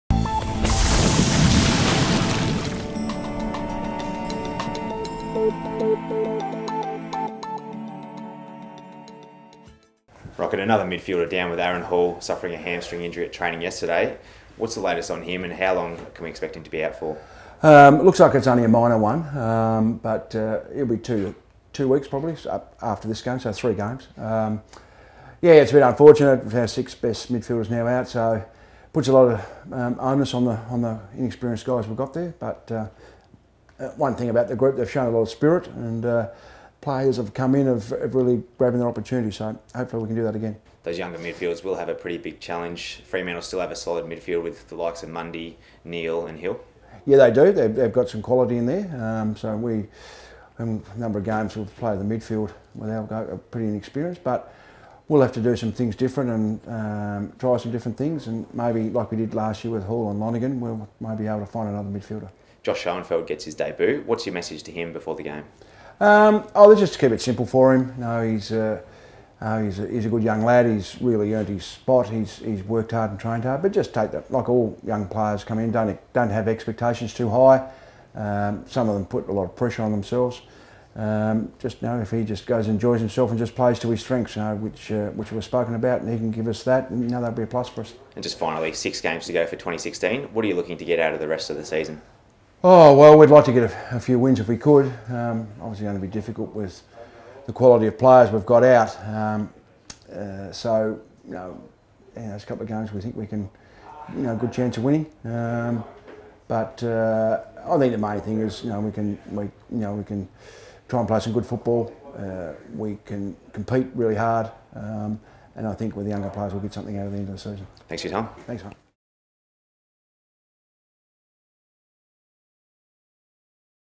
United Coaches Chat: Round 18
GC SUNS coach Rodney Eade speaks to SUNS TV ahead of the round eighteen clash against Fremantle.